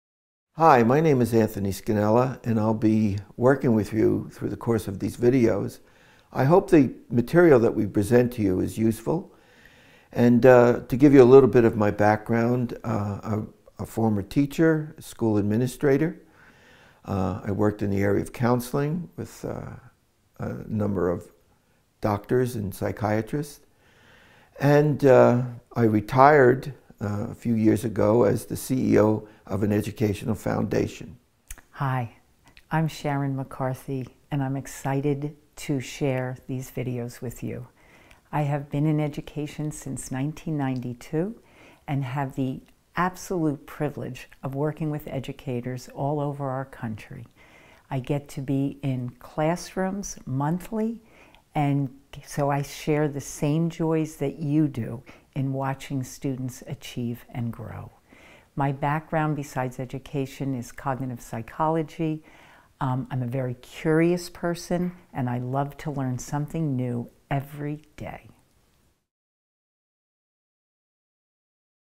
Introducing Tony and Sharon - EPI's Launch Your Classroom!
Our professional development materials are more than just books. In Launch Your Classroom, you will have access to exclusive video content of your authors and educational experts, Dr. Anthony Scannella and Sharon McCarthy. These videos enhance the material in this book, providing valuable resources, demonstrations, and opportunities for further learning.

▹ Launch Your Classroom! Series